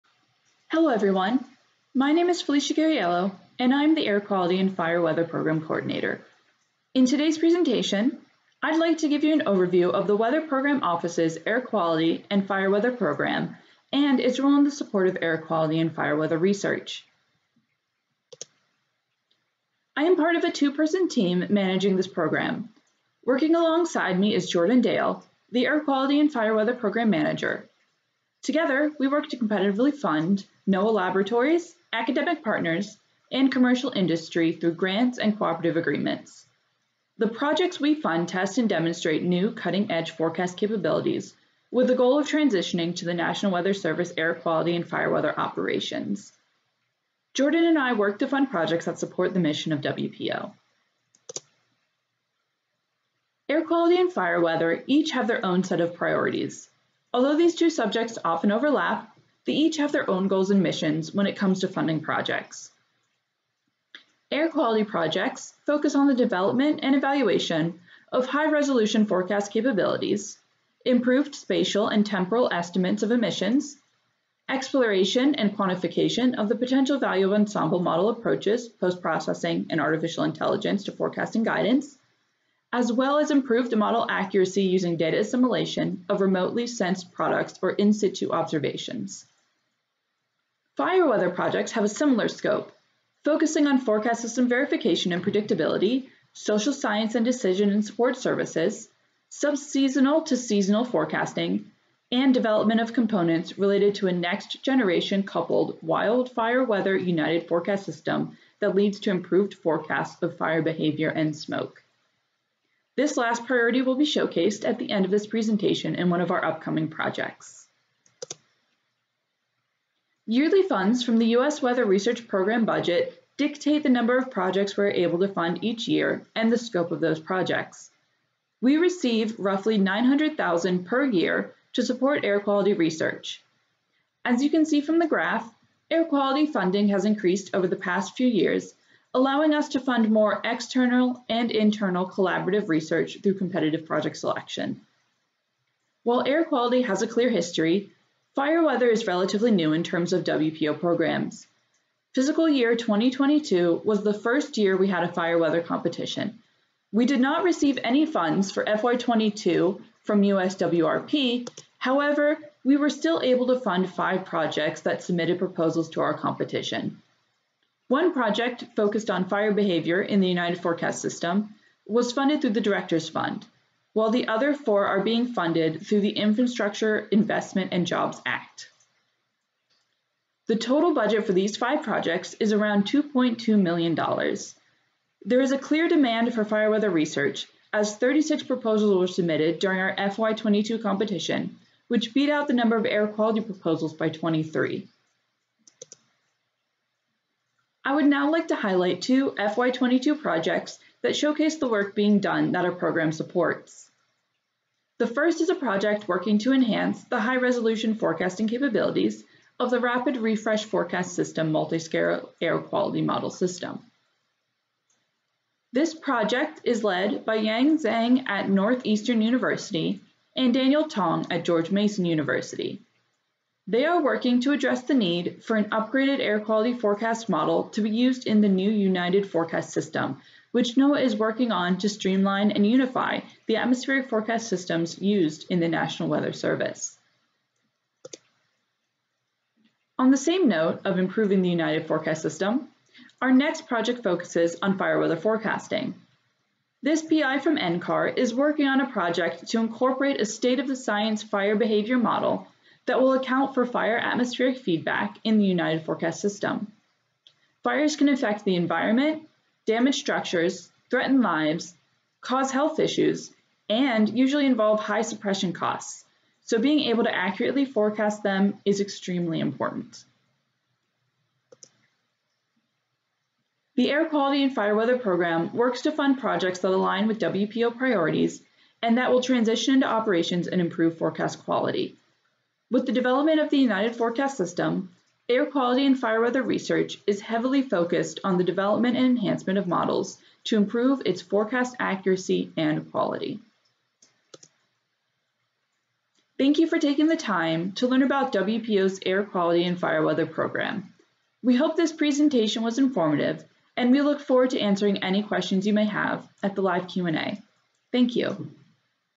NOAA Research's Weather Program Office Air Quality and Fire Weather Program
This video provides an overview of NOAA Research's Weather Program Office Air Quality and Fire Weather Program, given by Program Coordinator, Felicia Guarriello. This video was recorded for the Weather Program Office's 5-year Program Review in January 2023.